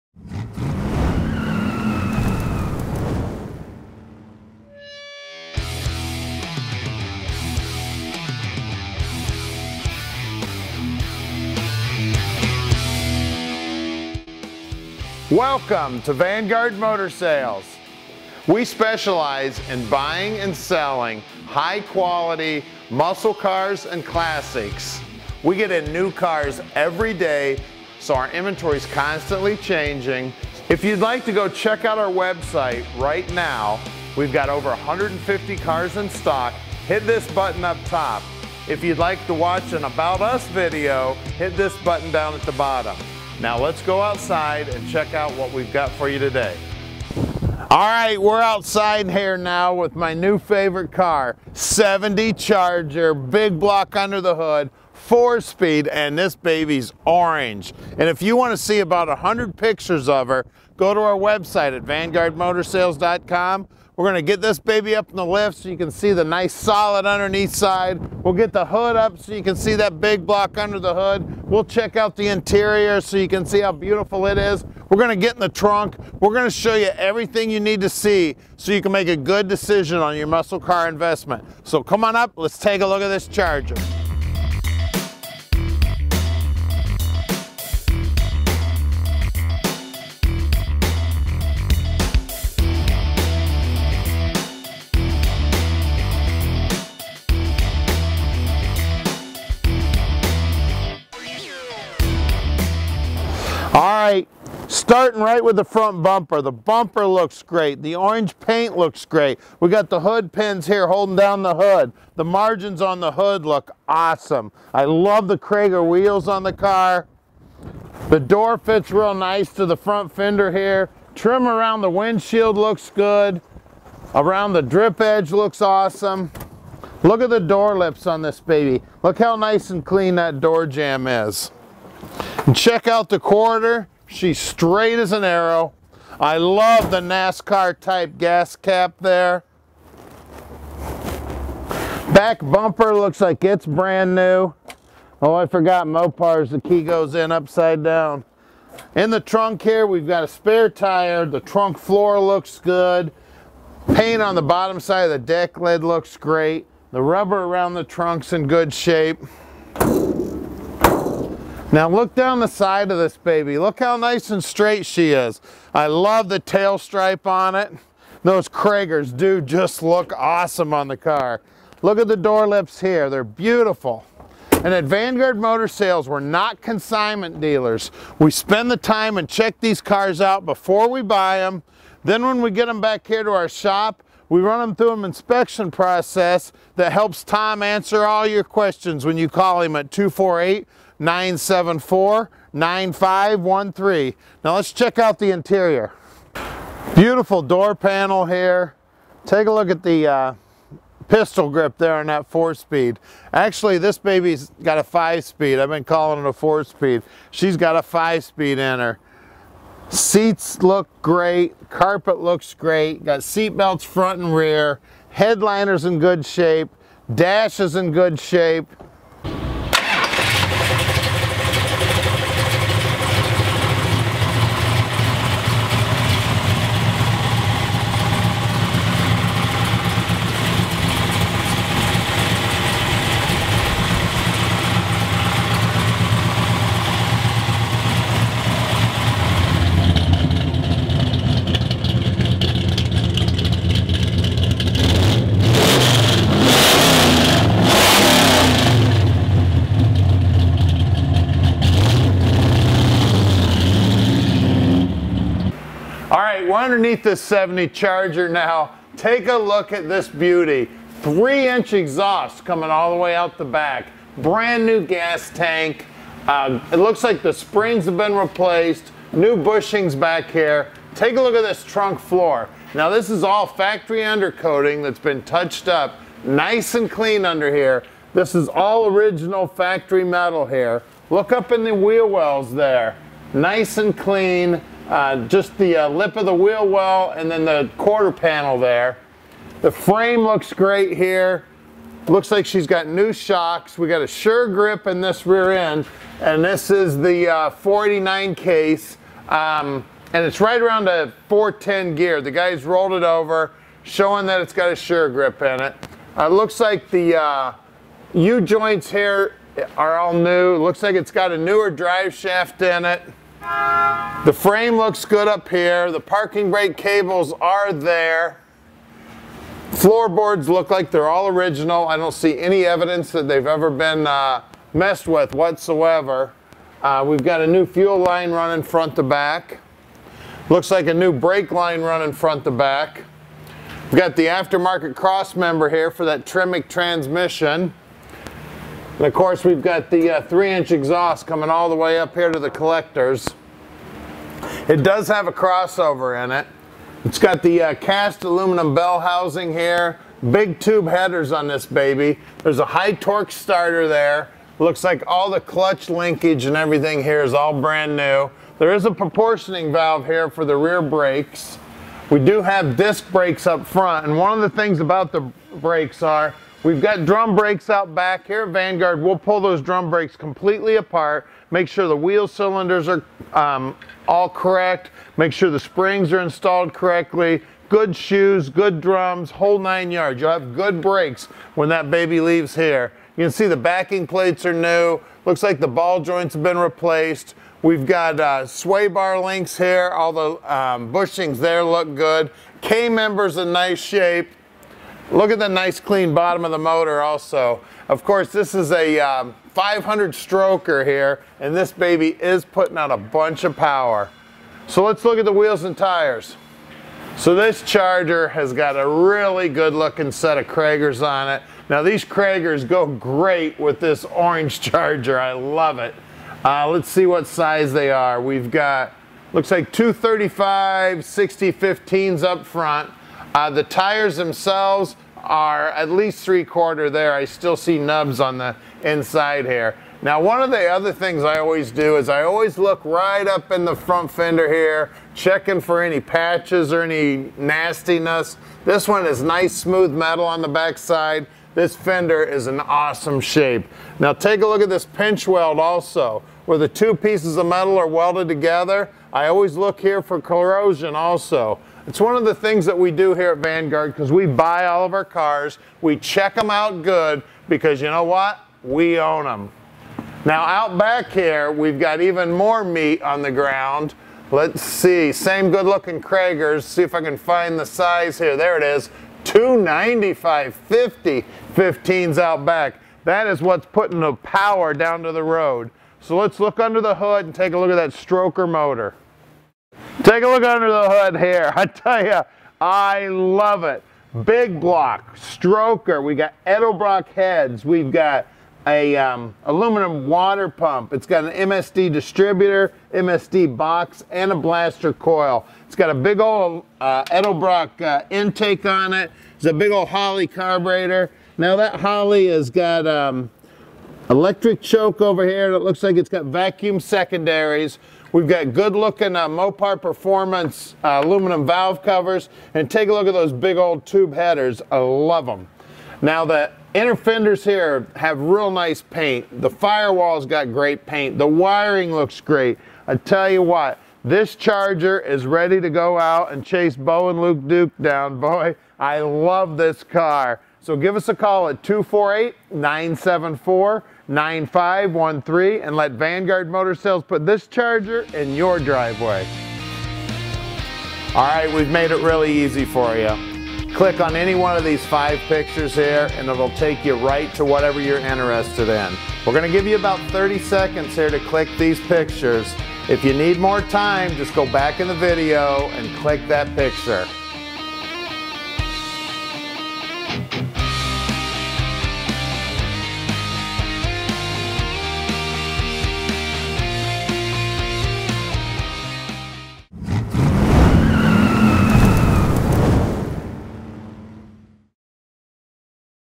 1970 Dodge Charger For Sale
1970 Dodge Charger 500

VIN:  XP29L0G162216

It’s known the ’68 ’69 and ’70 Dodge Chargers are one of the most sought after Muscle Cars available today. The unique looks and monster Mopar engines make these cars iconic. Vanguard Motors has been known to house some of the meanest and cleanest Mopars on the market and this Charger is no exception.

Starting with a clean body, this Charger was restored from the ground up. This beauty started life as a 383 Automatic car. The previous owner purchased the car in 1997 and at the time it sported a 440ci V8 and  4-Speed Manual Transmission. Lots of time and money was invested to make this ’70 Charger the dominating street machine it is today. This great looking Mopar is a strong, reliable driver! Less than 5,000 Miles since the complete restoration.

Documentation and Receipts from the restoration are included in the sale.

Mechanics

Professionally Built 500ci Stroker V8 Engine (440ci Mopar Based) with less than 5,000 Miles Since Built

Keith Black Hyper Eutectic Pistons

Edelbrock Performer RPM Heads

Edelbrock Intake Manifold

Mopar Performance Valve Covers

Roller Rockers

Mopar 509 Hydraulic Cam

Mopar Aluminum Water Pump

MSD Distributor & Blaster Coil

High Volume Oil Pump

Holley 750 Street Avenger Carburetor

Tremec TKO600 5-Speed Manual Transmission

Centerforce Clutch

8 ¾ Sure Grip Rear End w/ 4.10:1 Gear Ratio

TTI Ceramic Coated Headers

Power Steering

3” Dual Exhaust

New Driveshaft

Front Disc Brakes, Rear Drum – Manual Brakes

Stainless Steel Brake Lines

New Gas Tank

New Fuel Lines

All Wiring Replaced with Brand New Reproduction Harness from Year One

High Torque Starter

Brand New Gas Tank

Exterior

Car was Sanded to Metal and Painted “Hemi Orange” in 1997 – Paint looks Awesome

Original Sheet Metal is in Great Condition

Pinch Welds Look Phenomenal

White Tail Stripe looks Great

Front and Rear Bumper Chrome Looks Awesome

Hood, Doors, and Trunk Fit Great to the Body

Window and Drip Edge Trim look Great

Nice Paint on Underside of Decklid and Hood

Nice Rubber on Doors and Trunk

Doors Operate Awesome

Door Lips are Nice and Clean

Nascar Style Fuel Filler

New Trunk Floor

Frame Looks Awesome

Floorboards Look to be All Original with Factory Undercoating

K-Member in Great Shape

Wheel Wells are Nice and Clean

Cragar Wheels

Newer Kelly Charger Tires

235/60R15 Front

295/50R15 Rear

Interior

Gorgeous White Interior

Recovered Front and Rear Seats

Original Seat Backs and Seat Mechanisms

Beautiful Door Panels

Dash Looks Great

Pistol Grip Shifter

Seat Belts Front and Rear

Headliner Looks Awesome

Wood Grain Steering Wheel

New Carpet

Spare Tire

Trunk Mat

Call Tom 7 Days a Week…